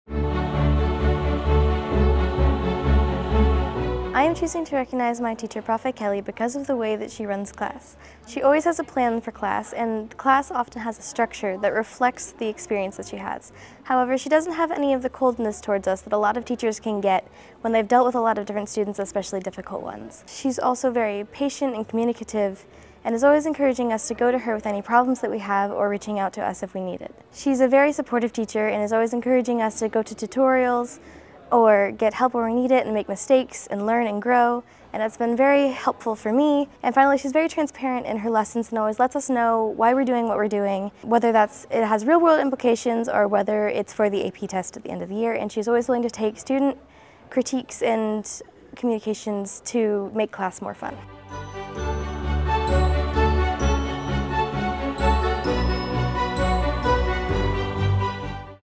LEEF STAR Award: Vista Ridge HS Jordyn May recognizing Kristen Kelly
The Leander Educational Excellence Foundation (LEEF) hosted its ninth-annual Student Teacher Achievement Recognition (STAR) Banquet April 23, 2024. At the banquet, students selected an educator who has had an impact on their educational journey.

In this video, Jordyn May from Vista Ridge HS recognizes Kristen Kelly.